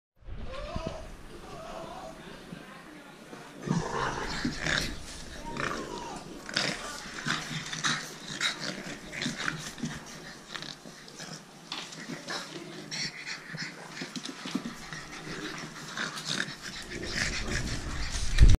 Precious Gems Frenchie pups
Storm and Tsunami playing with mum